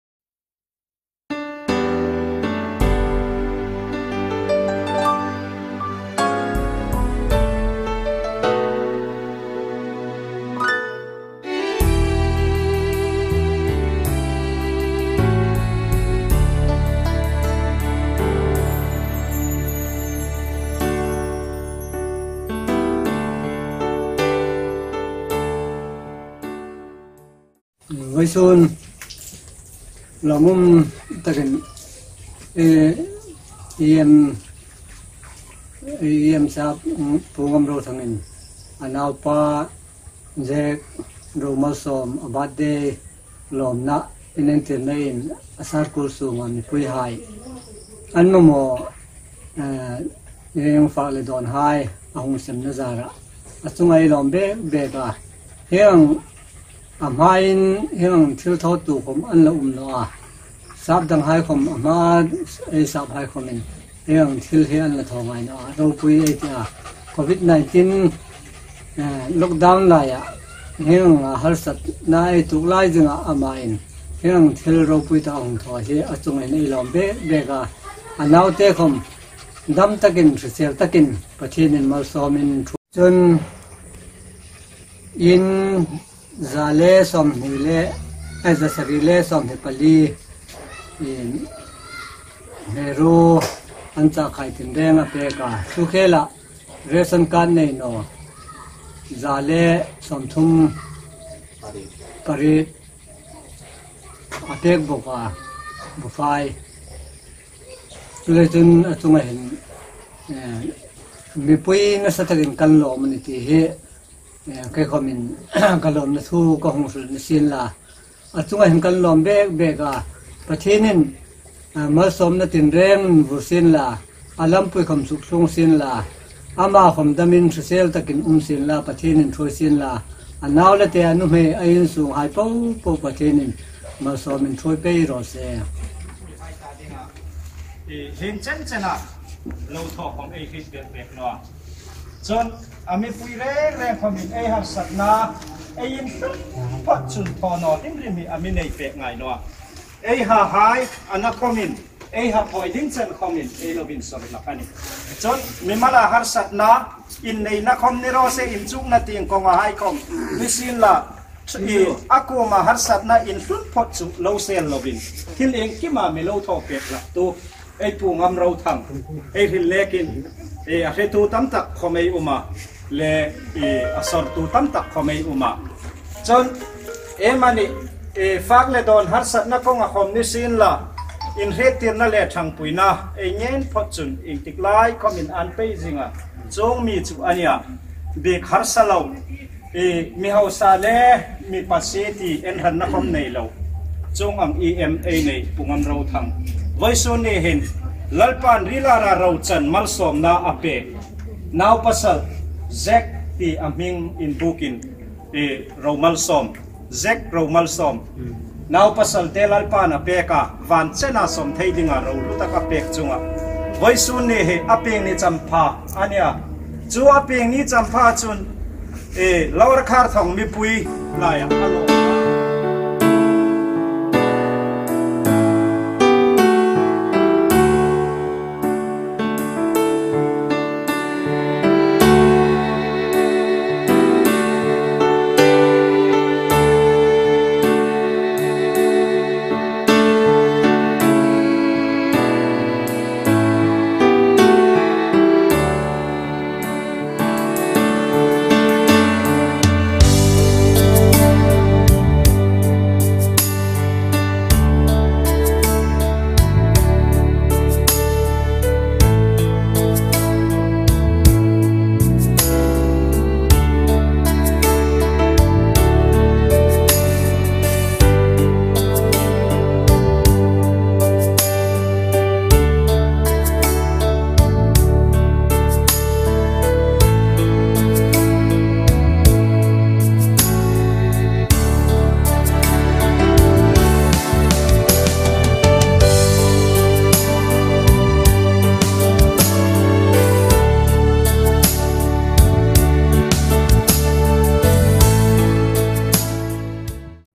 ZAC ROMALSAWM BIRTHDAY THILPEK LE LAWMTHU HRILNA.
ZAC ROMALSAWM HMAR BIRTHDAY THILPEK.

15th June 2021.

Pu Ngamrothang Hmar (EM NCHAC) le Pi Jeneth Hmar hai chun, vawisun an naupa Mr Zac Romalsawm Hmar kum khat pieng champha lawmna'n, Lower Kharthong Constituency sunga khuo popo a thilpek anin hlan.
Thilpek inhlan hai.
1. Tel (M.Oil)
2. Machi.
3. Neutrela.
4. Dal.
5. Purunsen (onion).
6. Alu (potatoe).
Hi thilpek hi In 724 (houses) hai kuoma inhlan a nih. Chun, Ration Card nei naw in 138 (houses) hai kuoma Bufai KG 20 seng apek bawk a nih.

Pu Lalramnghak (P.Hnachangzawl GB) in, Pathien kuoma lawmthu hrilna Covenant School a neiin, thilpek hai hi Harangajao area, Michikhur area le Kayeng Valley tieng po a sem ding hai thawn suok a nih.
P.Hnachangzawl khuo mihai chun Covenant School a lawmna thilpek hi an hung lak seng a nih.

Hieng ang hunkhir/hun harsa lai tak khawm nisien Birthday thilpek a biel mipui hai an hlan hi mipui an lawm hle. Mr Zac Romalsawm Hmar ta dingin ṭawngṭaina nei pek ei tiu a, Lalpa Pathien in veng zing sien, hriselna le varna inthuom rawseh. Petu hai Pathien in a let tamin malsawm bawk rawseh.